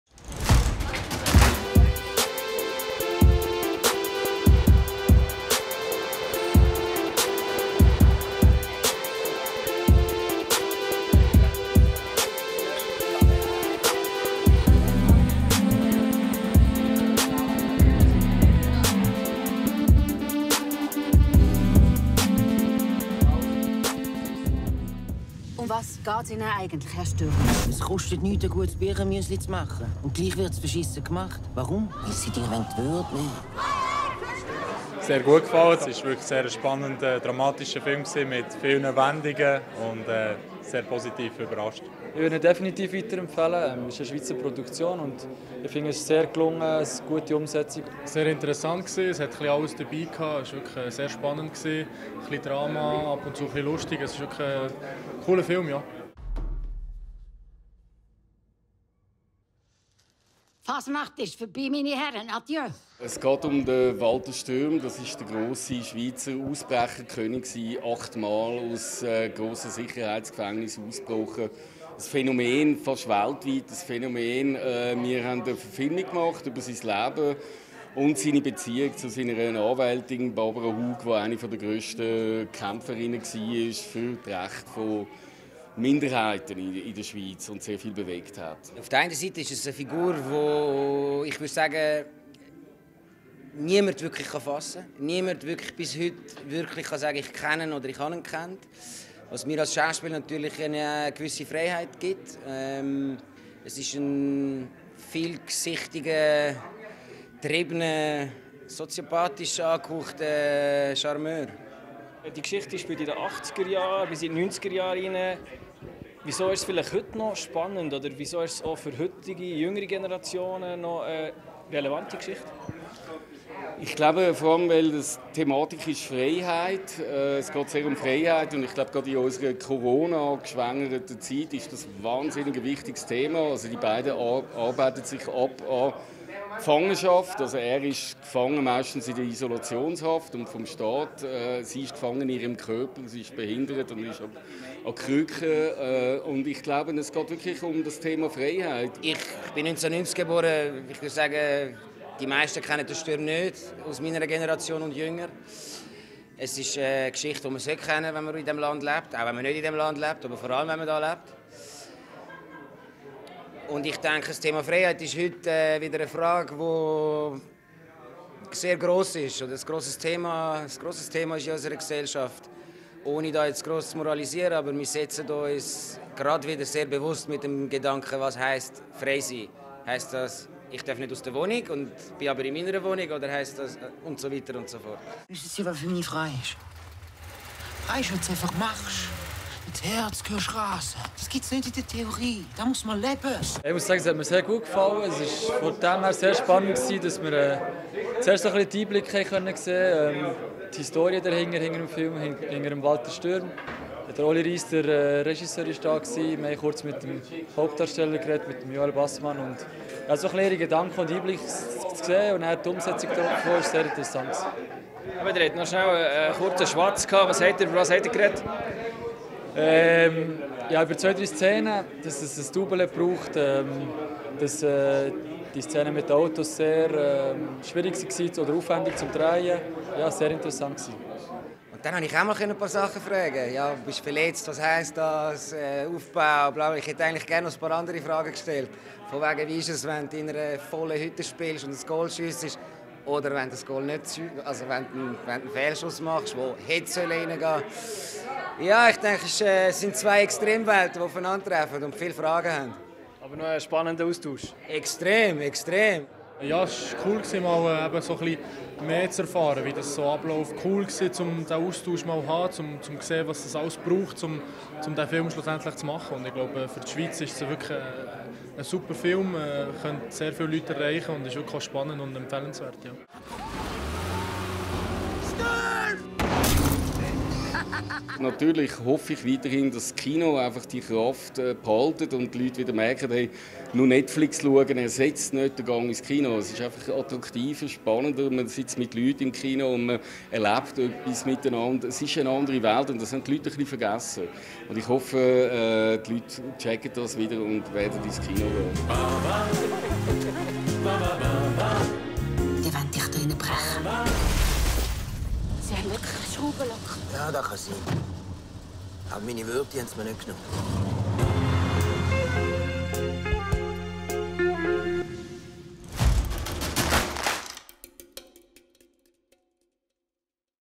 Grosse YB-Delegation an der «Stürm»-Filmpremiere
Am Donnerstag ist der Film «Stürm – bis wir tot sind oder frei» im Ciné Club in Bern angelaufen. Bei der Premiere des von Regisseur Oliver Rihs produzierten Films war eine grosse YB-Delegation dabei. Oliver Rihs ist der Sohn des 2018 verstorbenen YB-Aktionärs Andy Rihs.

Nach der Premiere erfuhren die YB-Spieler David von Ballmoos, Nicolas Bürgy und Cédric Zesiger von Hauptdarsteller Joel Basman und Oliver Rihs Details aus erster Hand.